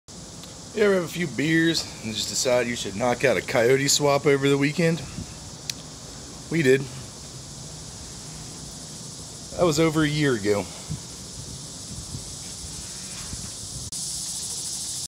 1966 Mustang 5.0l Coyote Swap
Progress on the 66' Mustang Coyote Swap. It's been a rough run, but here is hoping she drives soon. Video's coming to the channel soon